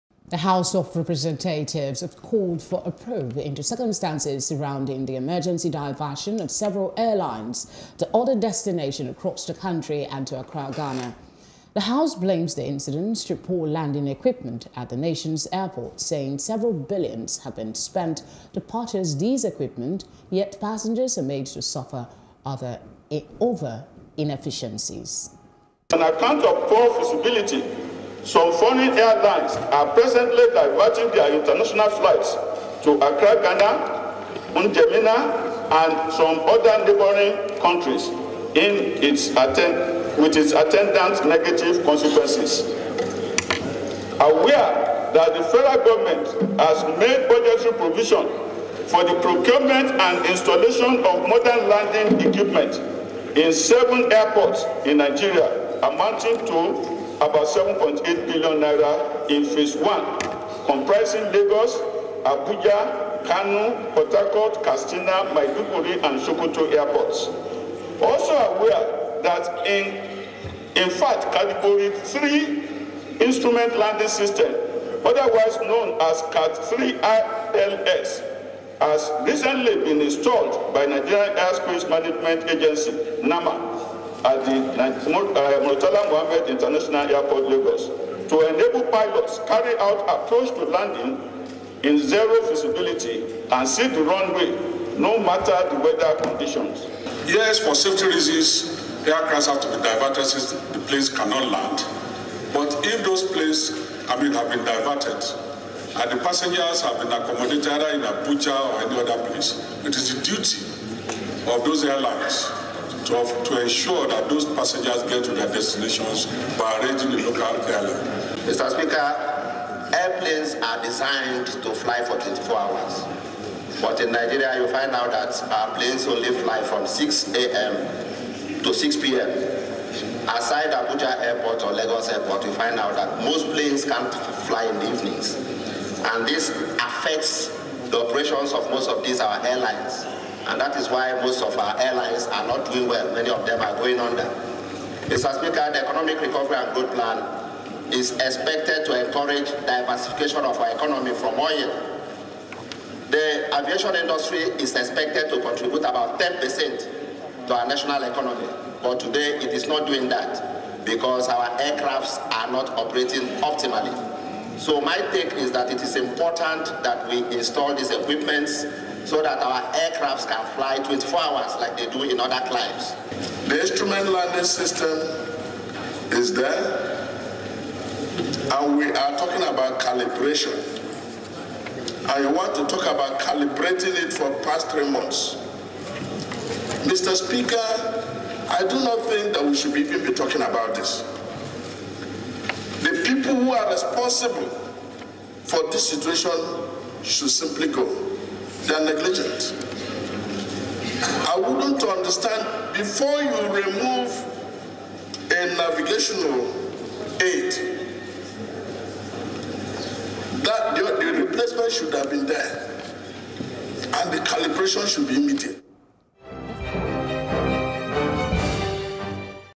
Poor Landing Facilities: Reps Wants Federal Agencies Probed Over Flight Diversions
The House of Representatives has called for a probe into circumstances surrounding the emergency diversion of several airlines to other destinations across the country and to Accra Ghana.